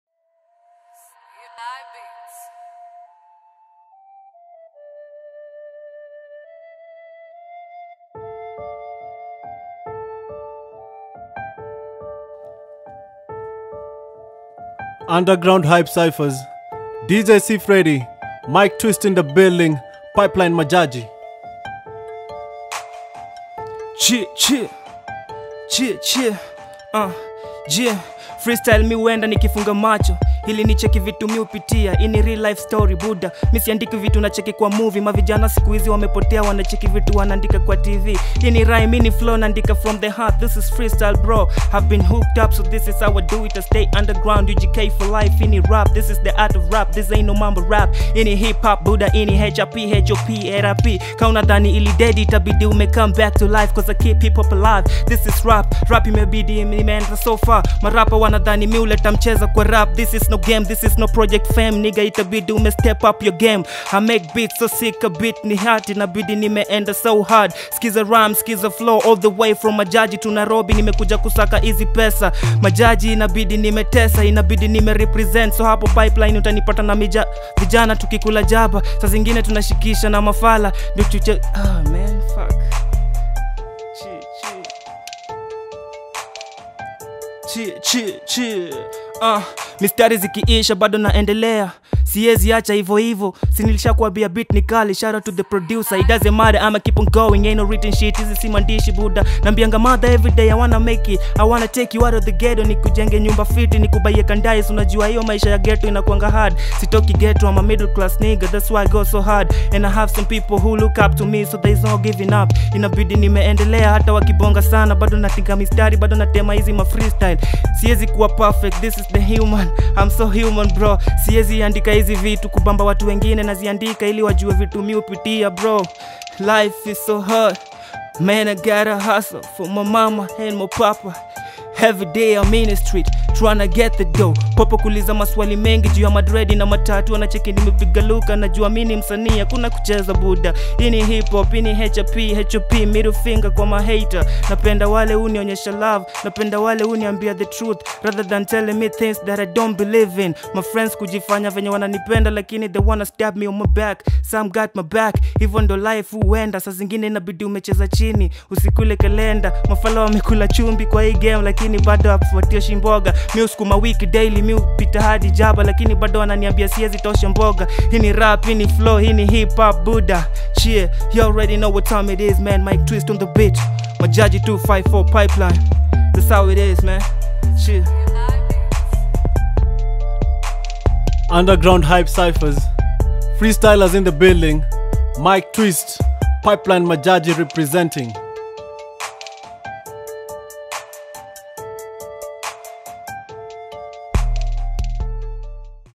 MYQ TWEEST | UNDERGROUNDHYPEKe | FREESTYLE RAP 2018
Myq Tweest is an Underground artist in Kenya, from Pipeline. Watch his freestyle!!